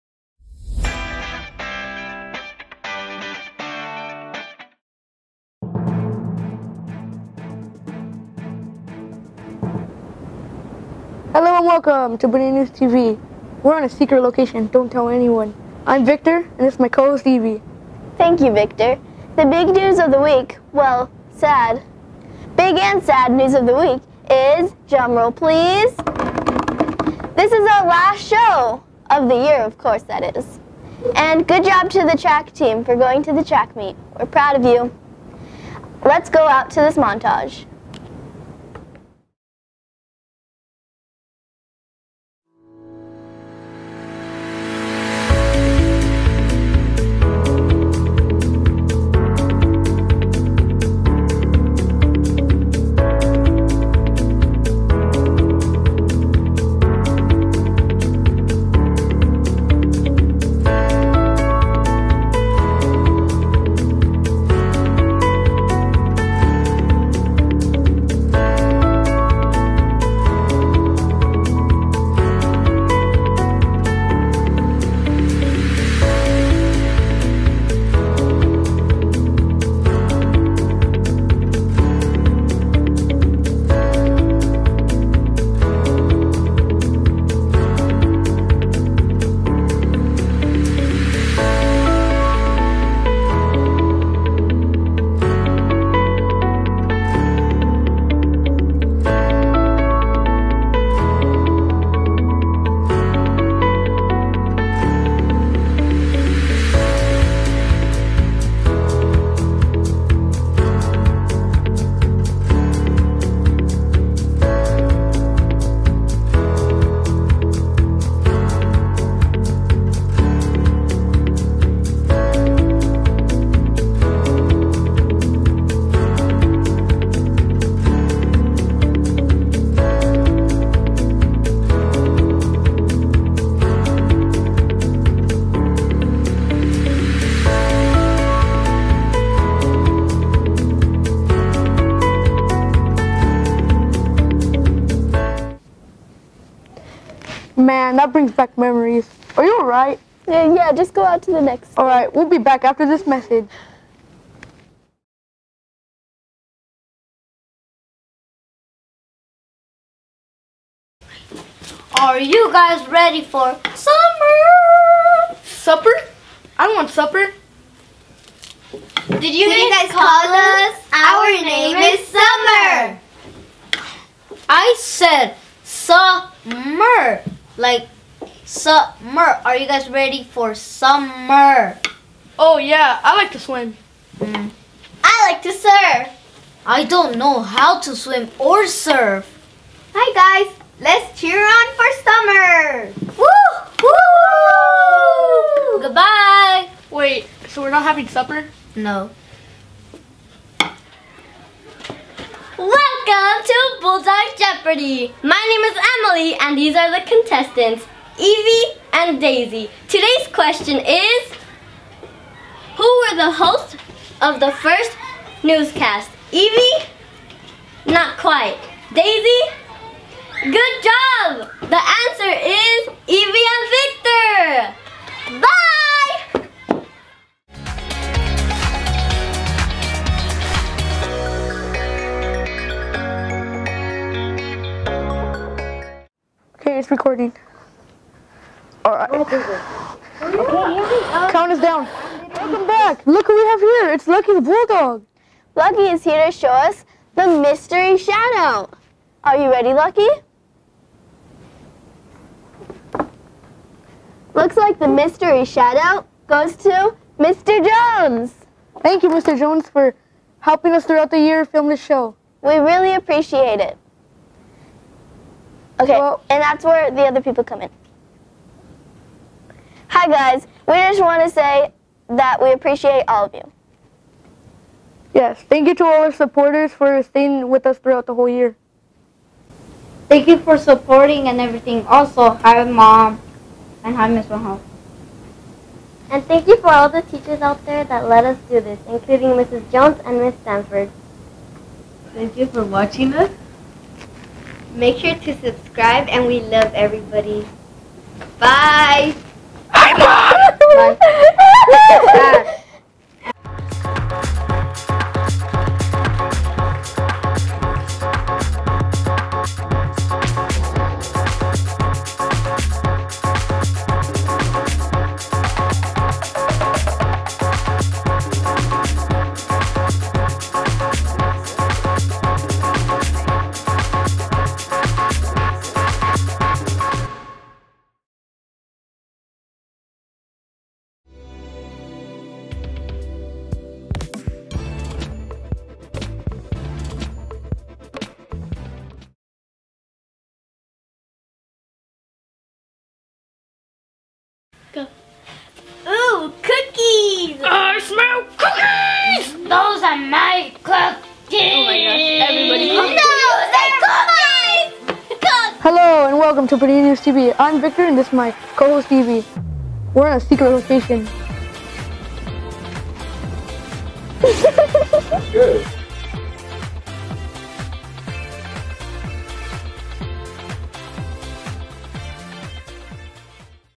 Bonita News TV - Final Show (18-19 Season)
Bonita News TV (BNTV) - Final Show 18-19 Season
Final show of the season the students say thank you to everyone. Please help support our new podcast for next year